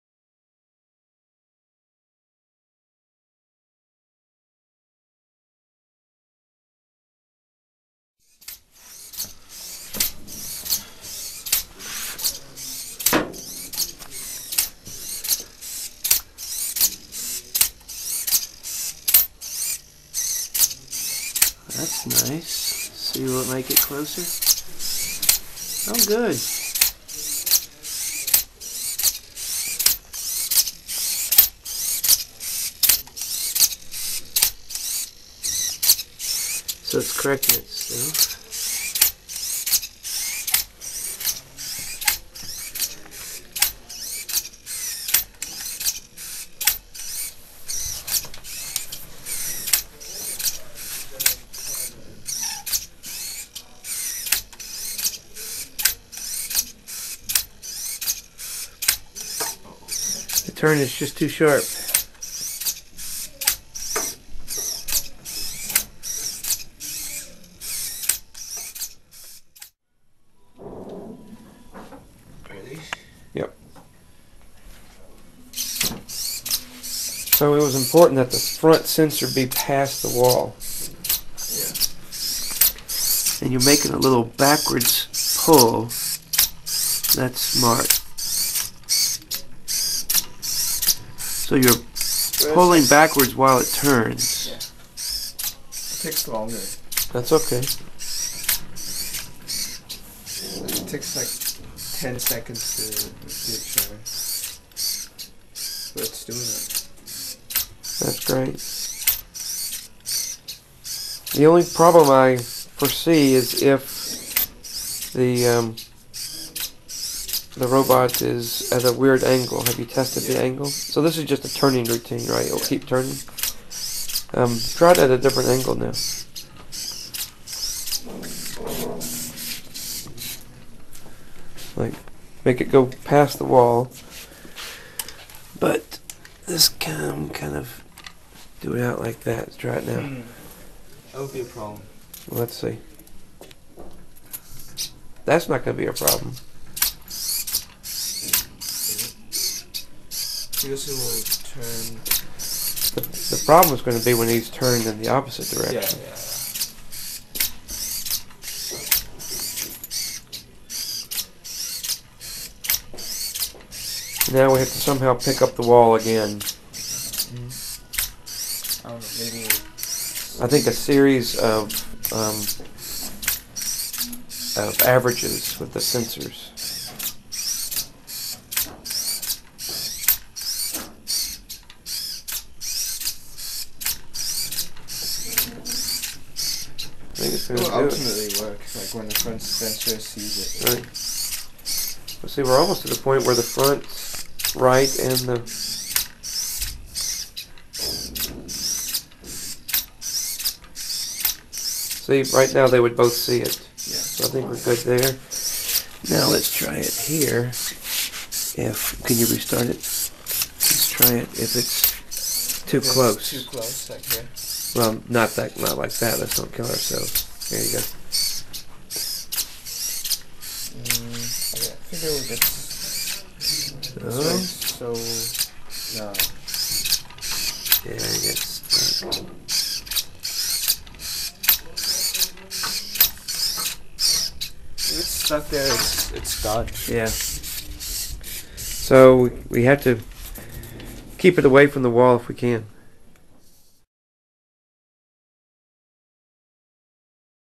Flash Firefighting Robot Part 2 -- George School 2009
Three of my students (Chun Wang, Checkie Chu, and Eric Engelhardt)  have modified a Robodyssey Roach robot for the International Firefighting Competition at Trinity College in Hartford, CT.  They call their robot Flash.  This is the first time weve entered a walking robot.  Eric and Checkie test their wall-following and turning algorithms.

Robodyssey Systems manufactures the Roach robot and RAMB II motherboard you see here.  The robots brain is NetMedias BX-24 microcontroller, which is programmed using the BasicX language.  I am the author of the worlds only BasicX textbook; if you are interested in learning how to program your own robot, see my